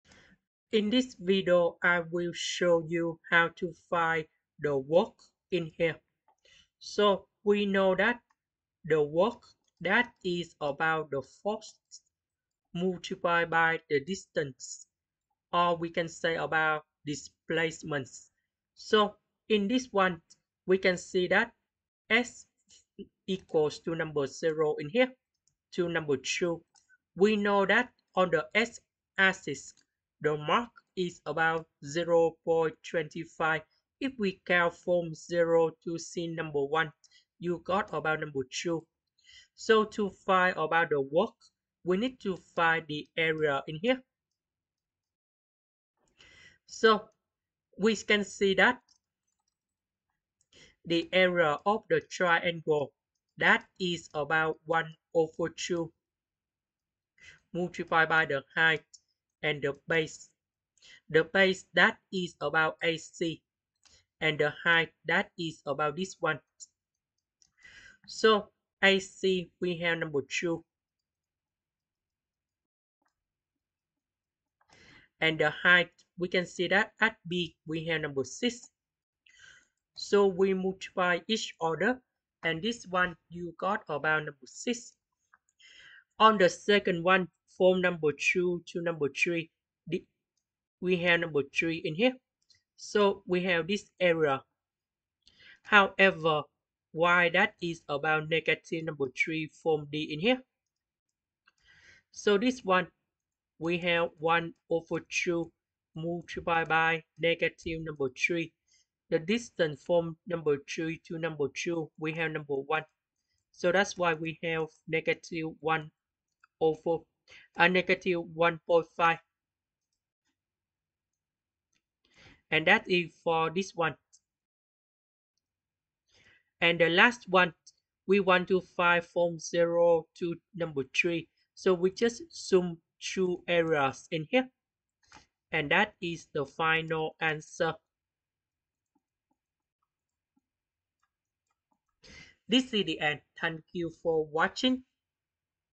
Physics Help: The force acting on a particle varies as in the figure below. (The x axis is marked in
Here is the technique to find the Word = Force x Displacement based on the graph